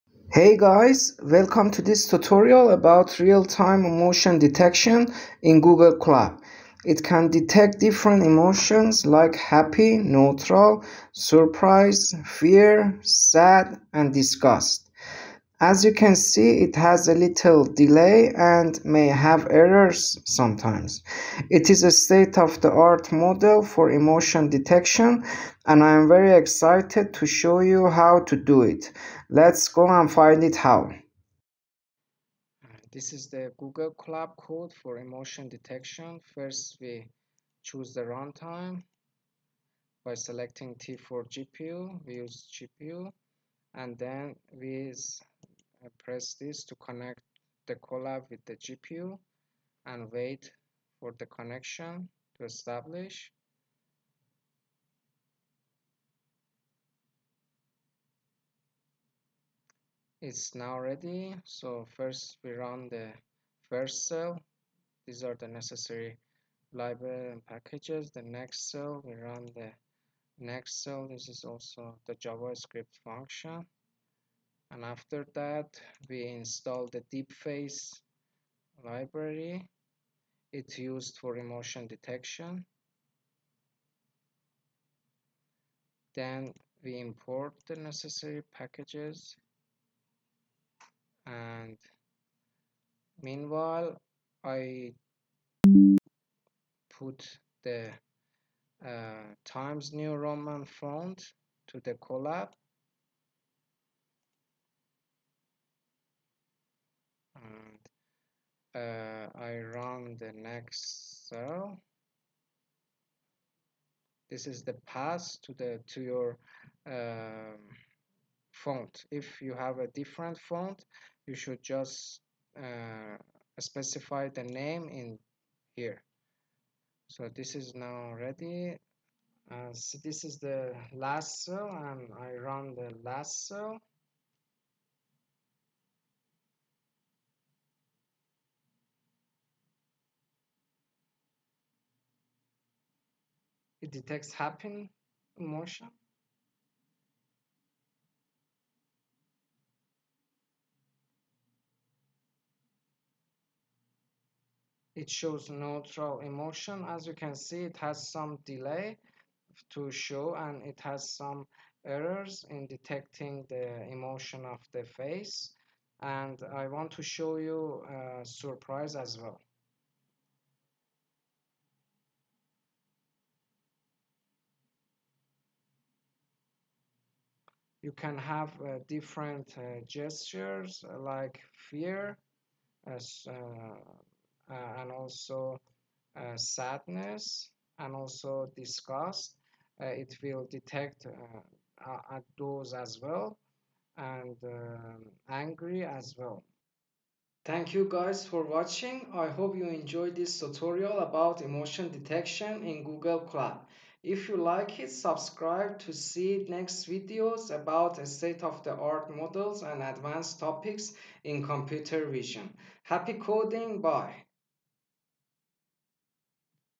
Real Time Emotion Detection in Google Colab
Curious about Real-Time Face Emotion Detection? 🤔
Discover how to detect emotions in real time with DeepFace using Google Colab’s free GPU! 🚀
Check out the latest research on this model on ResearchGate and start building your own emotion detection system today!

Chapters:
00:00 – Introduction
00:31 – Apply Deepface
02:11 – Real Time Emotion Detection